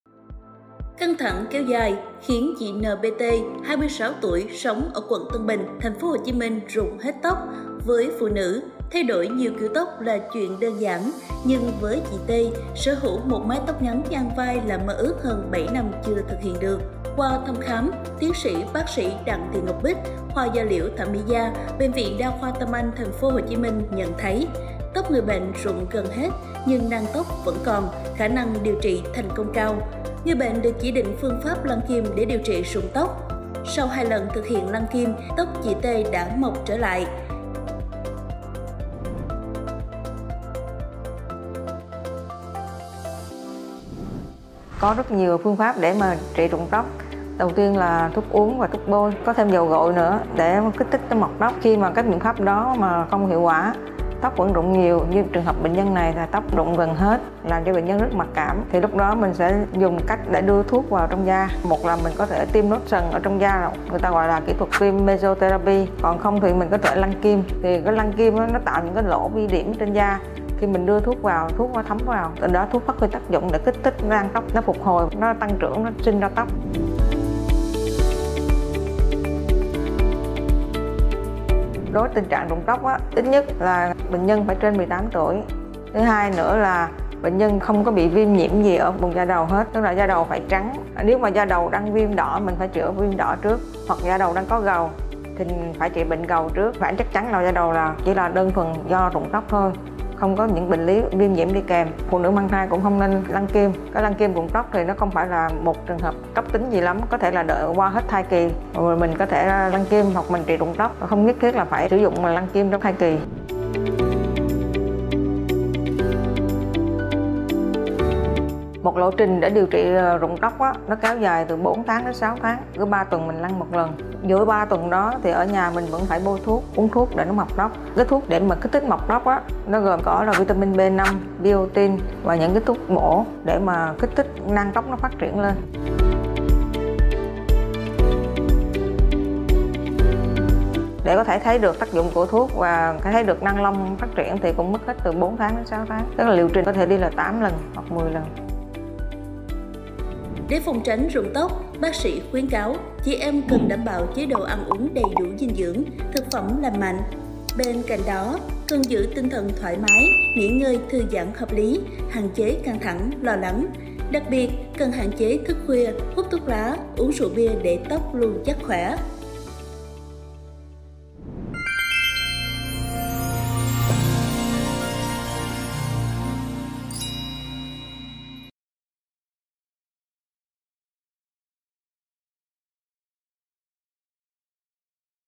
Hành trình tìm lại mái tóc sau 7 năm bằng phương pháp lăn kim | TS.BS Đặng Thị Ngọc Bích | Tâm Anh
💢 Căng thẳng kéo dài khiến chị N.P.T. (26 tuổi, quận Tân Bình, TP.HCM) rụng hết tóc. 7 năm chạy chữa, tóc mọc lại sau 2 lần lăn kim. Sợi tóc đen dày, dài hơn 10cm và phủ hết da đầu. Hiện chị đã tạm biệt mái tóc giả theo mình nhiều năm.

🧑‍⚕️ TS.BS Đặng Thị Ngọc Bích, khoa Da liễu - Thẩm mỹ Da, BVĐK Tâm Anh TP.HCM cho biết: có nhiều phương pháp điều trị rụng tóc gồm thuốc uống, thuốc bôi, dầu gội đặc hiệu… Khi các biện pháp trên không có hiệu quả, sẽ chọn phương pháp đưa thuốc vào trong da bằng cách lăn kim. Phương pháp này giúp thuốc thấm vào dễ dàng hơn, kích thích nang tóc phục hồi, giúp tăng sinh trưởng.

Mời quý vị tìm hiểu về “Hành trình tìm lại mái tóc sau 7 năm” qua video trên đây của TS.BS Đặng Thị Ngọc Bích.

🏥 HỆ THỐNG BỆNH VIỆN ĐA KHOA T M ANH